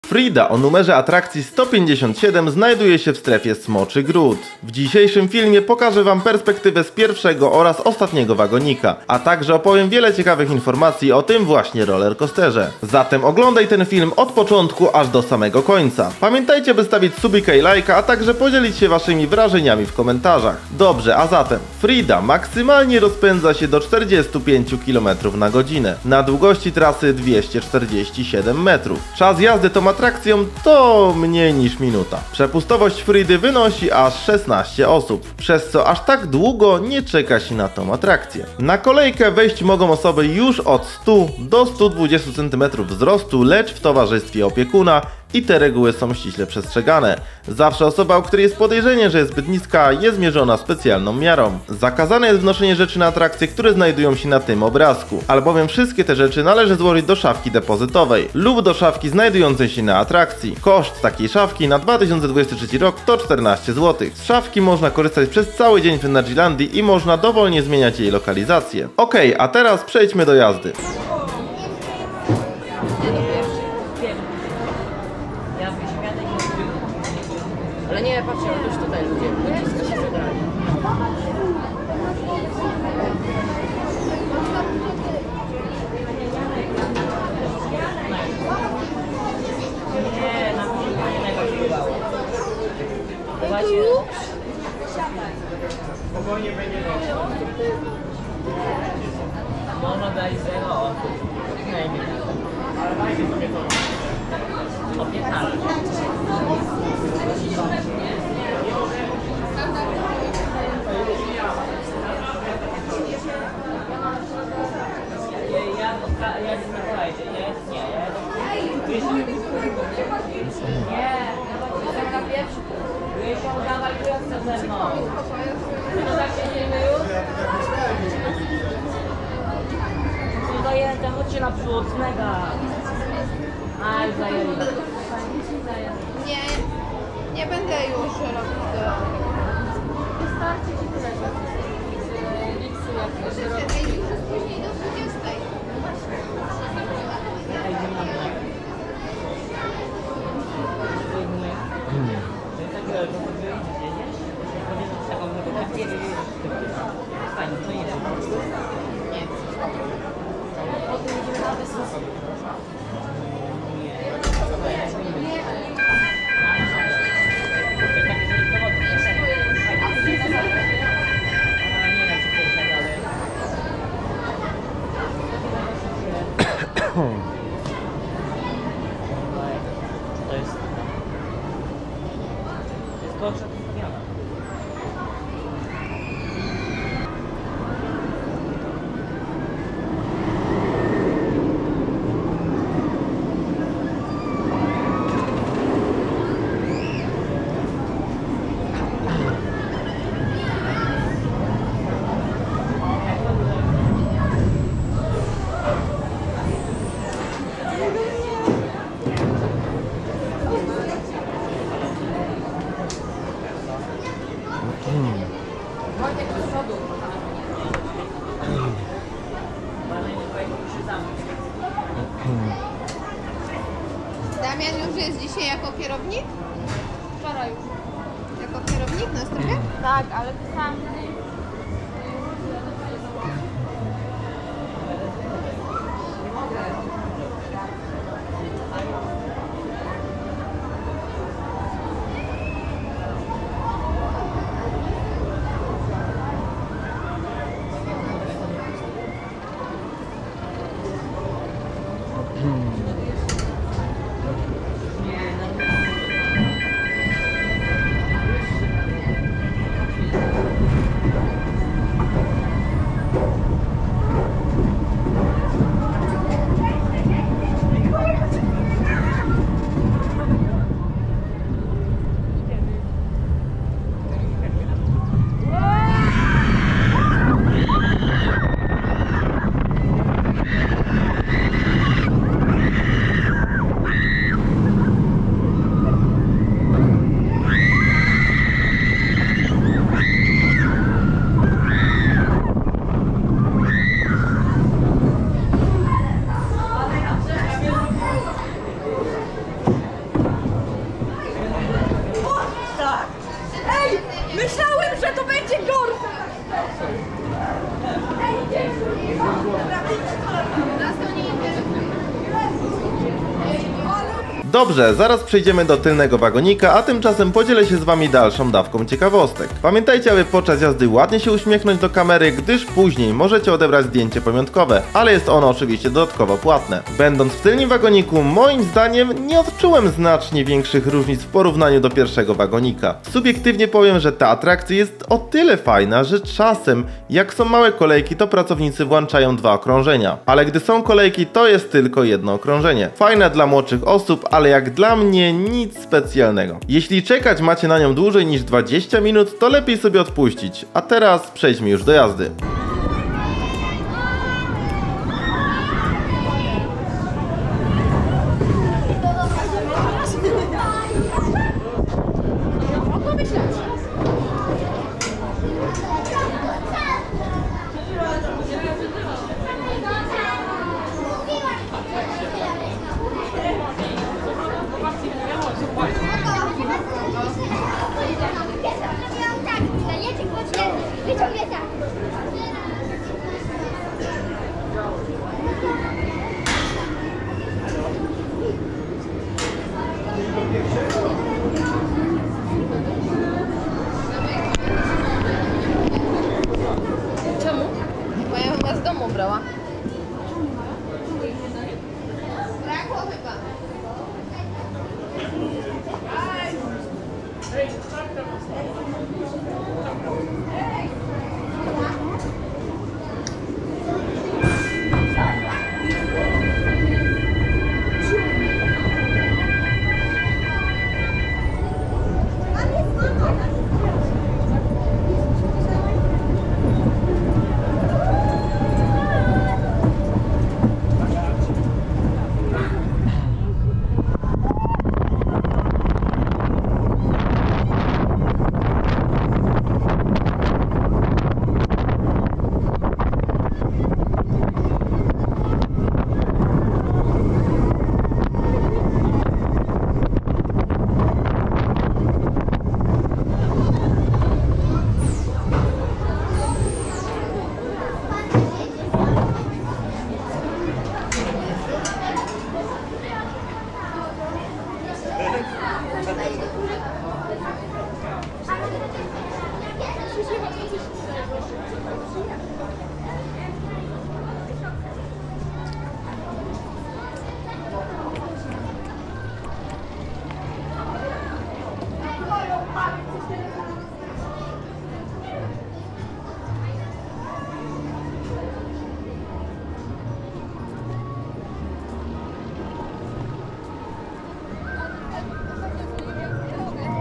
Frida - Energylandia 2023 (4K) (POV) (Zator, Polska) GoPro HERO 12
Hej w dzisiejszym filmie przedstawię wam 🎢 Frida - Energylandia 2023 (4K) (POV) (Zator, Polska) GoPro HERO 12. Energylandia ❤️ to Park Rozrywki największy w Europie położony w miejscowości Zator w Polsce. Na jego powierzchni znajdują się atrakcje takie jak Hyperion, Zadra, Abyssus, Mayan, Ekipa Light Explorers, Formuła, Energuś, Wonder Wheel, Park Wodny oraz wiele innych RollerCoasterów 😊 Jest to wymarzone miejsce do spędzenia wakacji 🏝️

▶️ 00:00 - Frida (Ciekawostki + Wejście do Atrakcji)
▶️ 01:10 - Frida (Pierwszy Wagonik)
▶️ 05:14 - Frida (Ciekawostki)
▶️ 05:52 - Frida (Ostatni Wagonik)
▶️ 08:07 - Frida (Fotobudka)

🎥 Filmy nagrywam kamerką: GoPro HERO 12 w pierwszym i ostatnim wagoniku w rozdzielczośći 5,3K i wyeksportowane jako 4K.